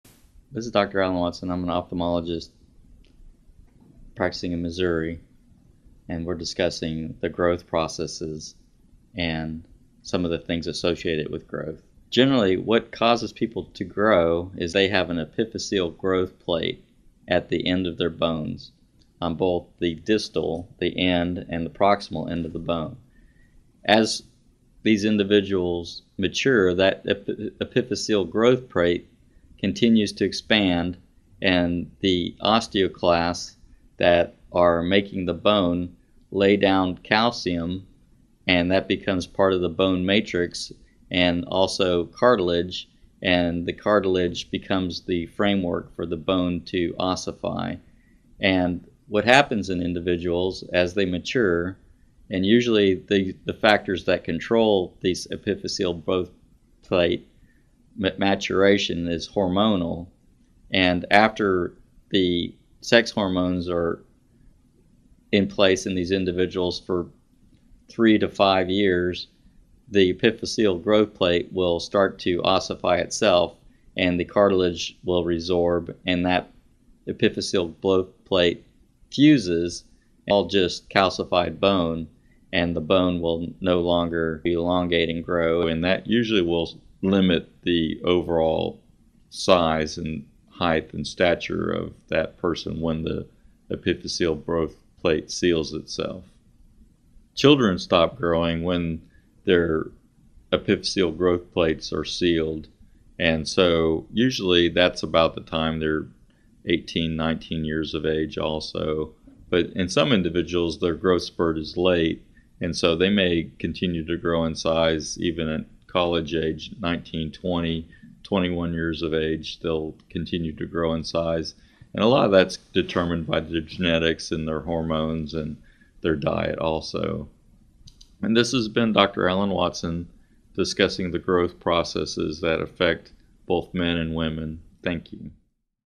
Human Growth Facts : When Do Children Stop Growing?
Children stop growing when their epiphyseal growth plates are sealed around 18 or 19 years of age. Learn how genetics, hormones and diet can play a role in when children will stop growing with information from an ophthalmologist in this free video on the human body.